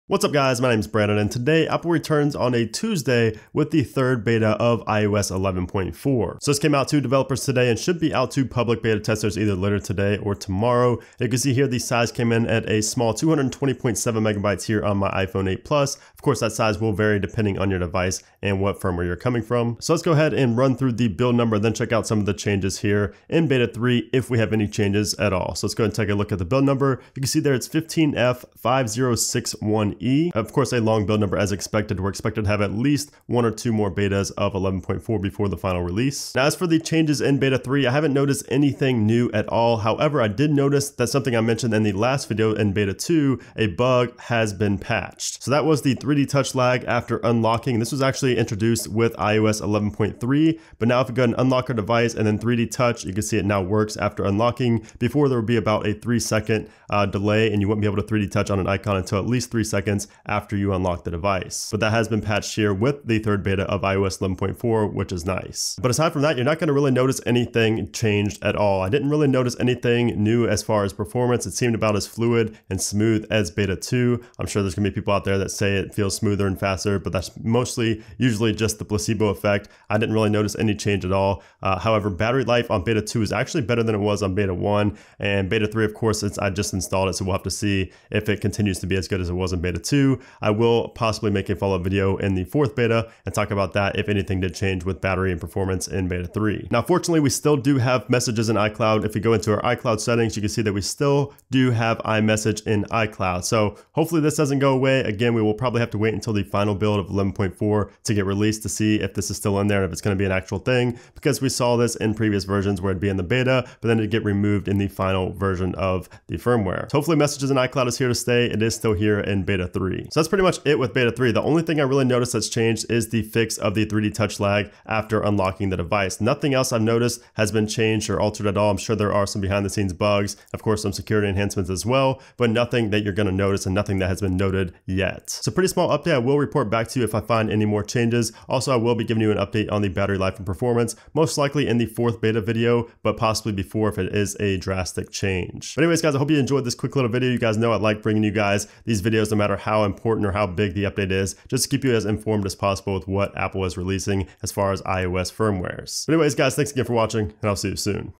iOS 11.4 Beta 3 Released - What's New?
Apple released iOS 11.4 Beta 3 today and in this video, we take a look at what's new, if anything.

Thoughts on 11.4 beta 3?

▬ STAY UP-TO-DATE! ▬